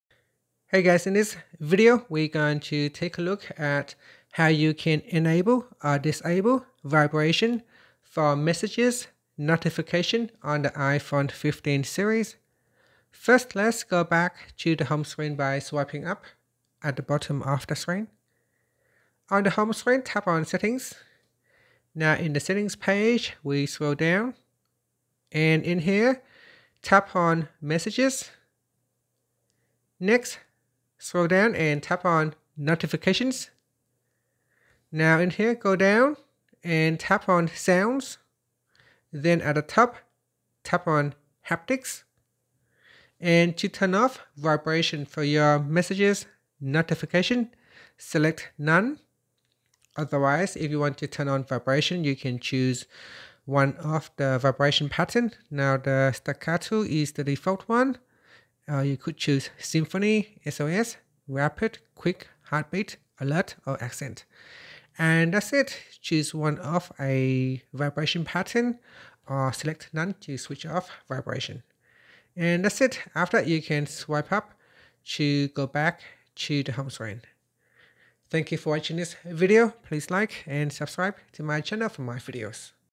iPhone 15/15 Pro Max: How to Enable/Disable Text Message Vibrations
In this guide, we'll explore how to enable or disable text message vibrations on your iPhone 15, iPhone 15 Pro, iPhone 15 Pro Max, and iPhone 15 Plus. Discover the purpose, benefits, and customization options that come with this feature, and learn how to tailor your iPhone experience to your preferences.

Benefits of Enabling/Disabling Text Message Vibration:

1. Silent Productivity: Enabling text message vibrations keeps you discreetly informed without causing disruptions in meetings or quiet environments.

2. Customization: You can personalize vibration patterns and intensity, making it easy to identify specific contacts or app notifications.

3. Battery Life: Disabling text message vibrations can help conserve battery life, ensuring your iPhone lasts longer between charges.

4. Accessibility: Vibration alerts are an essential accessibility feature for individuals who may not hear traditional notification sounds.

5. Peace of Mind: Control your notifications, minimize distractions, and maintain focus during important tasks.

In this video, we'll guide you through the steps to enable or disable text message vibrations, and provide tips on customizing this feature to enhance your iPhone experience. Whether you're a professional looking to stay focused or someone who values the customization options on their device, this guide will help you make the most of your iPhone 15 series.